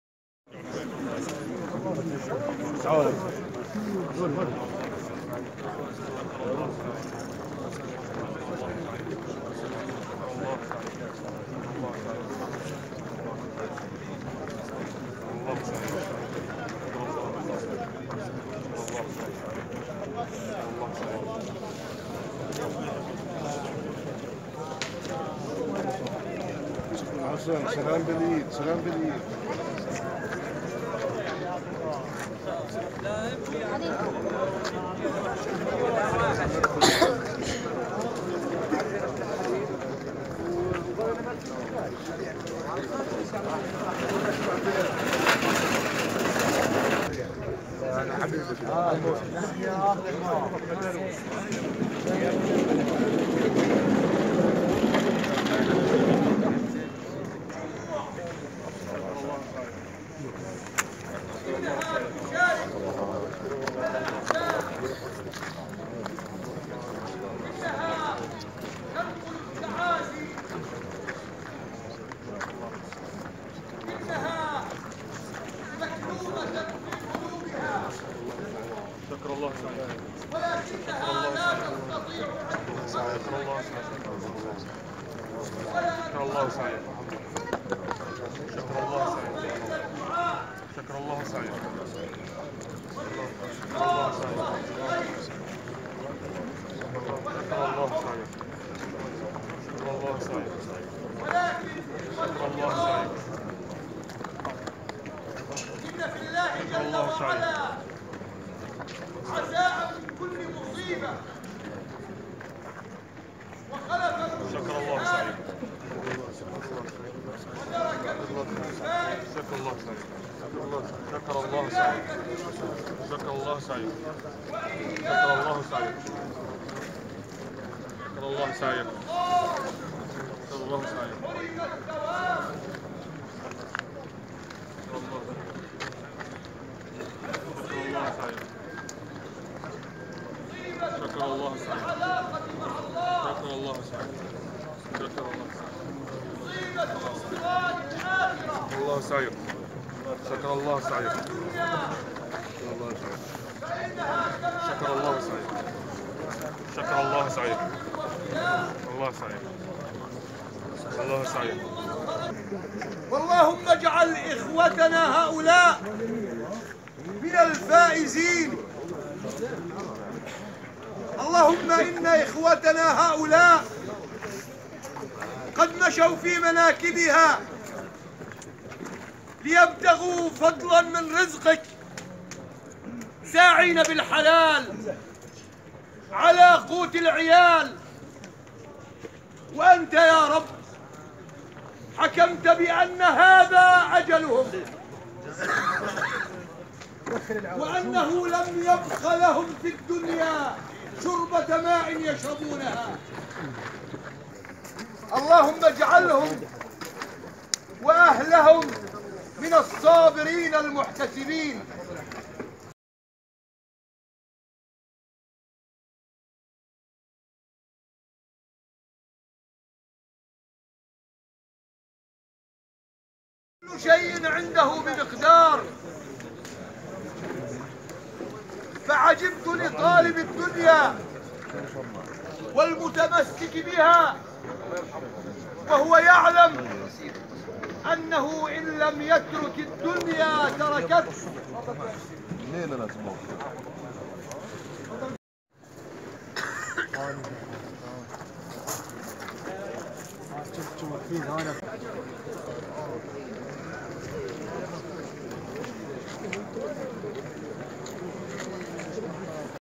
شاهد اعداد المعزين في وفاة الشقيقين علي ورزق ابو غنيم في بلدة الخضر
آلاف المعزين لذوي الفقيدين علي ورزق غنيم واللذين توفيا في حادثة سقوطهما عن عمارة قيد الانشاء في بلدة الخضر جنوب بيت لحم